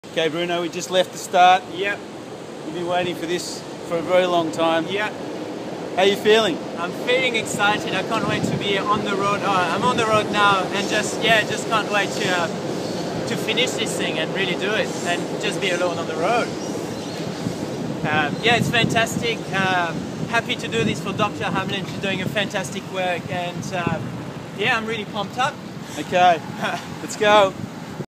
Cycling for Fistulas
Bruno is a nurse from Freemantle. He is riding solo from Sydney to Perth to raise funds for Hamlin Fistula Ethiopia, the Addis Ababa Fistula Hospital and the work of Dr Catherine Hamlin.